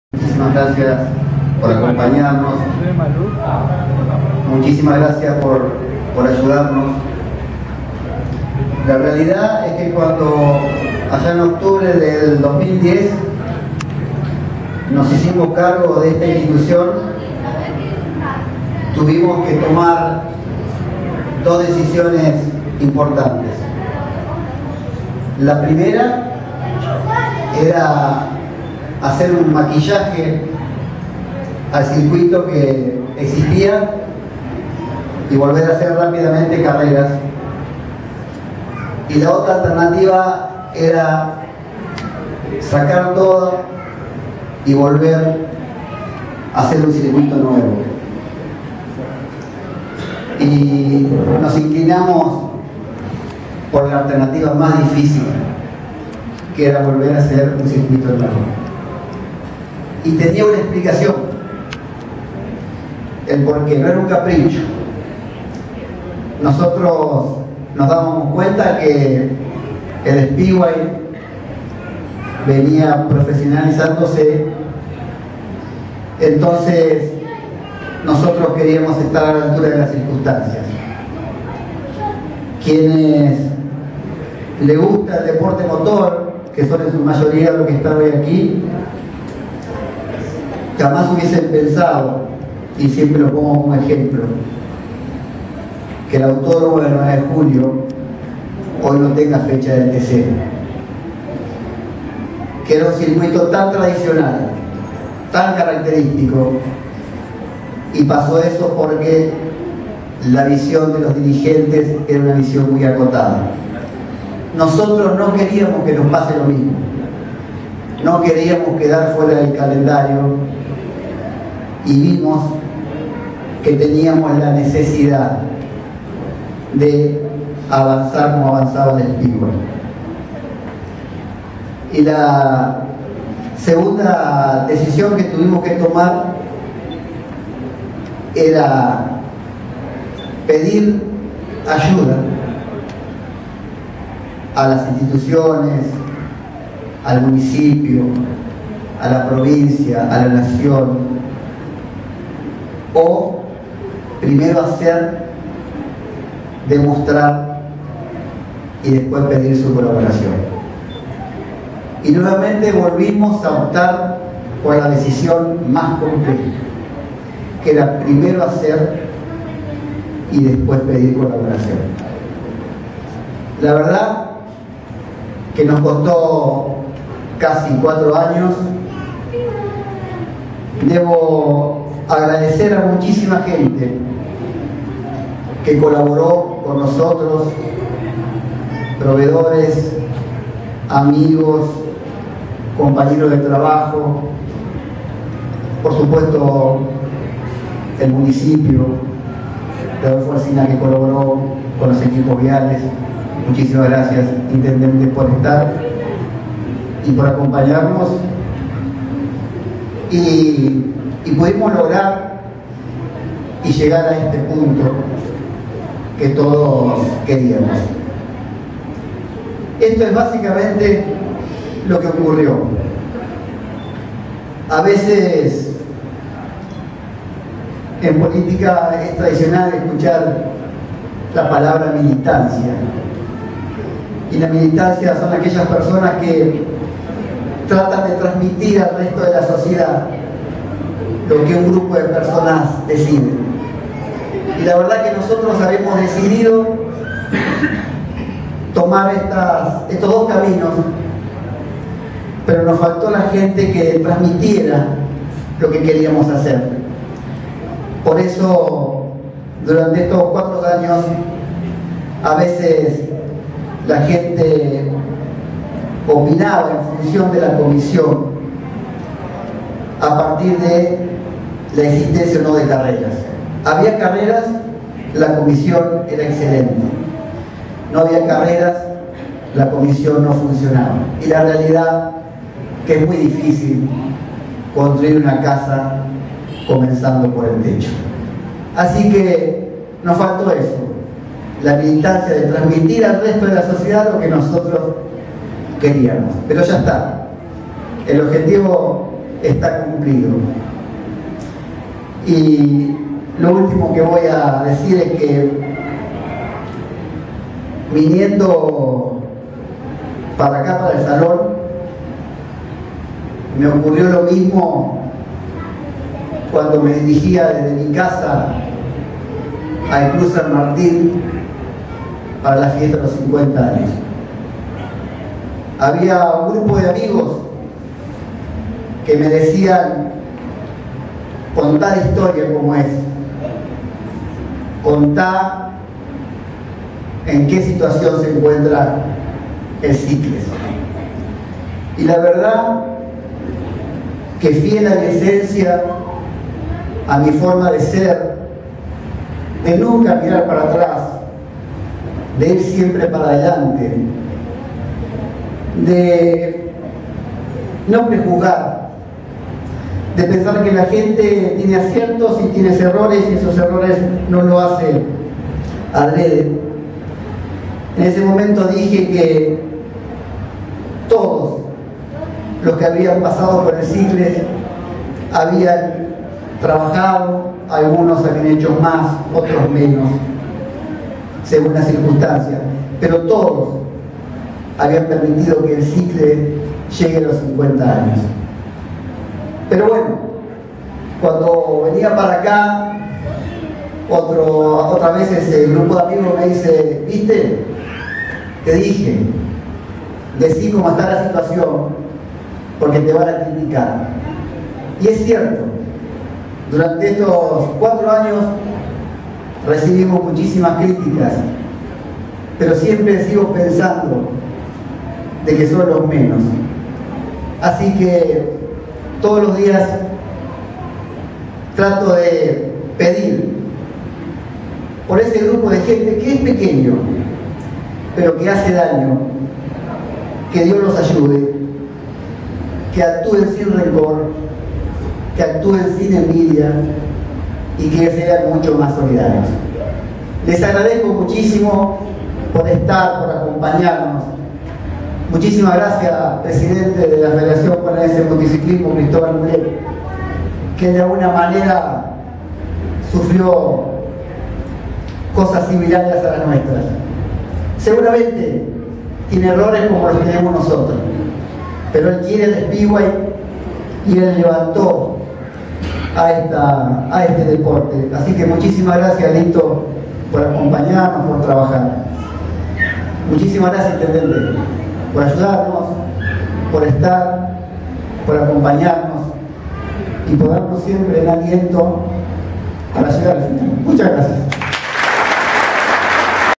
Presentación del Moto Karting Club
Acto de lanzamiento en Arroba eventos 23 de enero